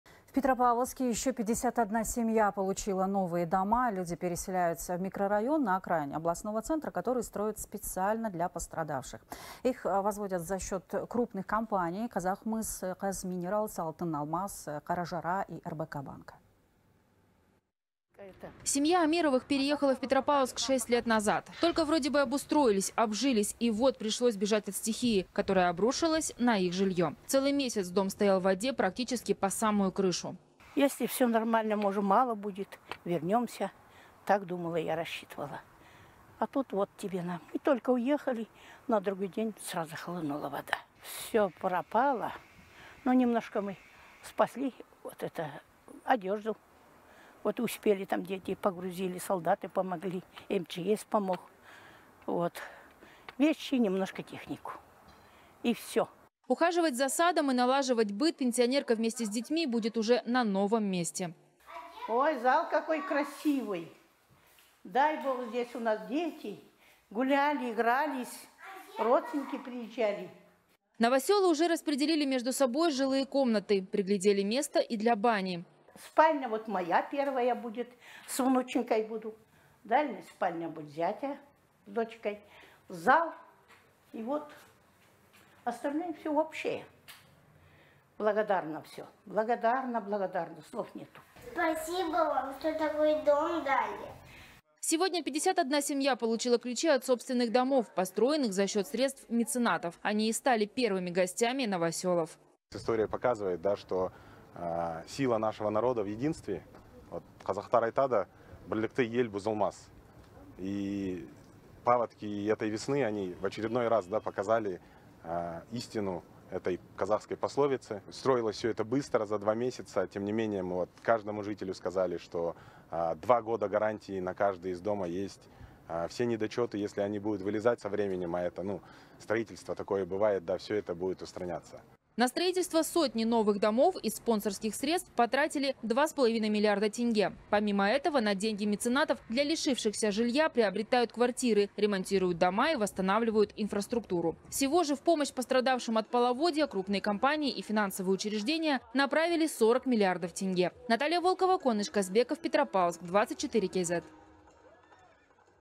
Пострадавшие от паводка петропавловцы получили новые дома
В Петропавловске еще 51 семья получила новые дома. Люди переселяются в микрорайон на окраине областного центра, который строят специально для пострадавших. Их возводят за счёт крупных компаний – «Казахмыс», «KAZ Minerals», «Алтыналмас», «Каражыра» и «RBK Bank», передаёт корреспондент «24KZ».